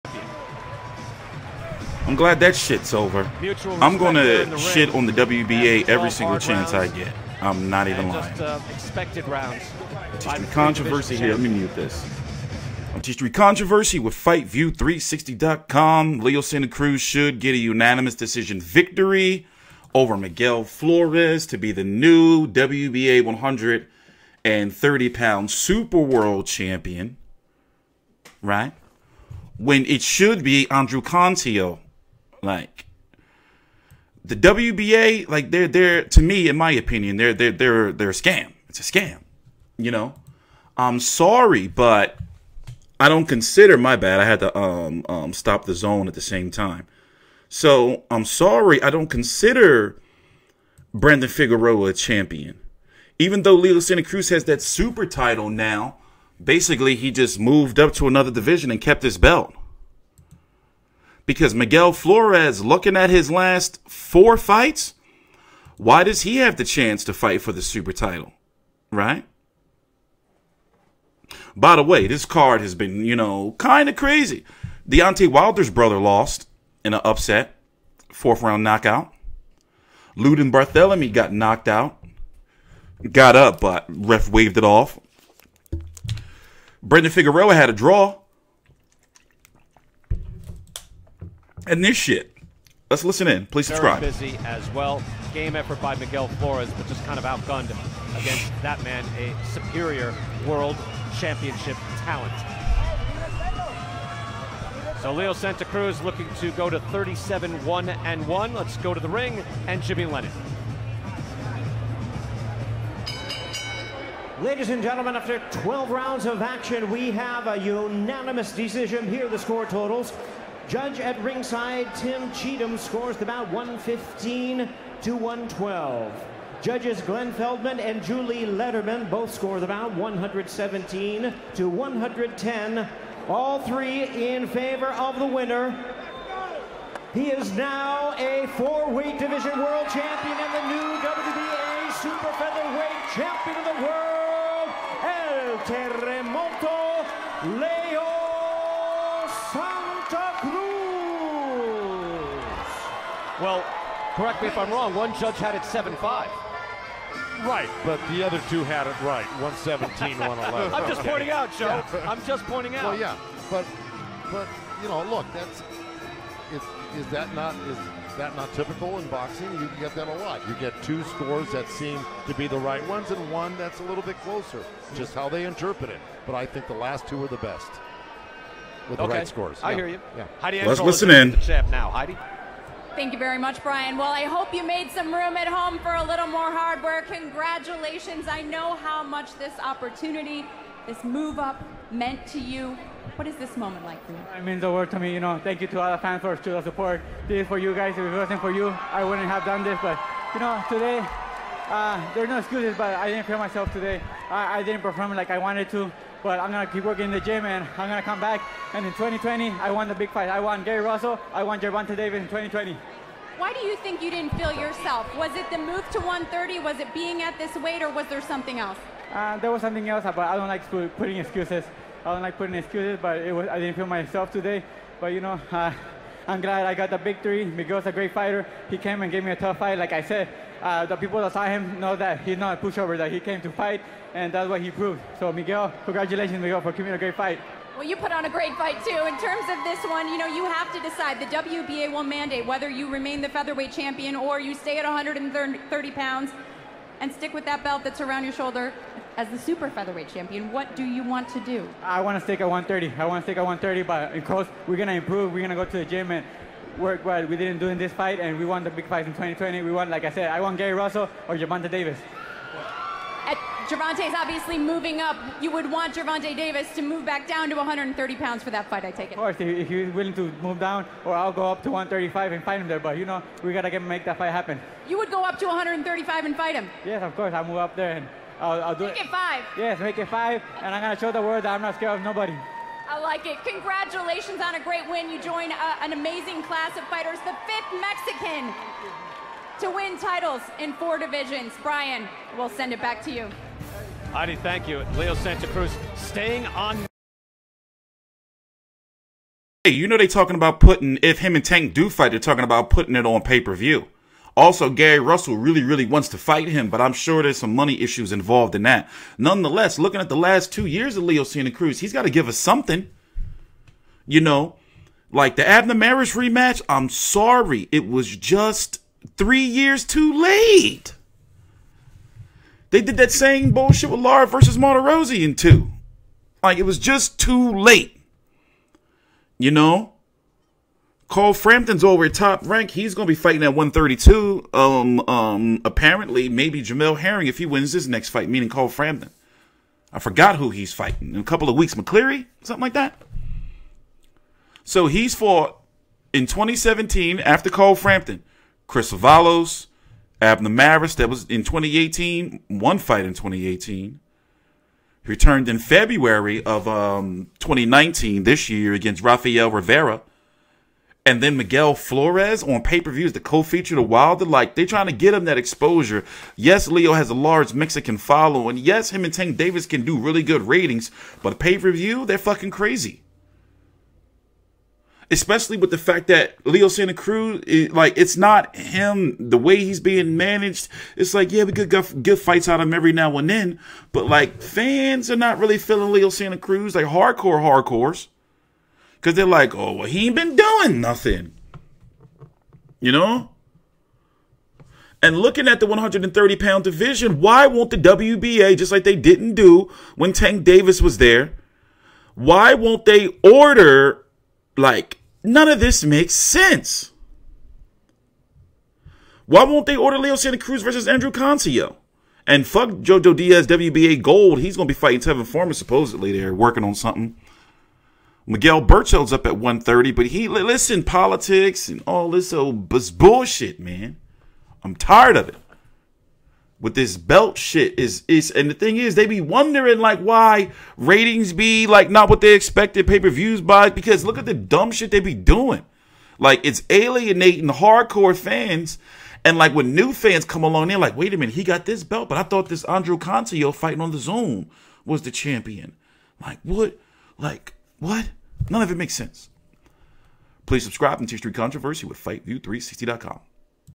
Leo Calls Out Tank & Russell! Santa Cruz vs Flores Results & HIGHLIGHTS! WBA TRASH Belt!
- Boxing FightView360 FULL Post Fight Results, Highlights, Recap, Full Fight Analysis, Post Fight Interview & More

FOR OUR NEW FV360 Merch! Also PLEASE Check:// 👈 for latest boxing news, rankings, schedules, highlights, reviews, & results **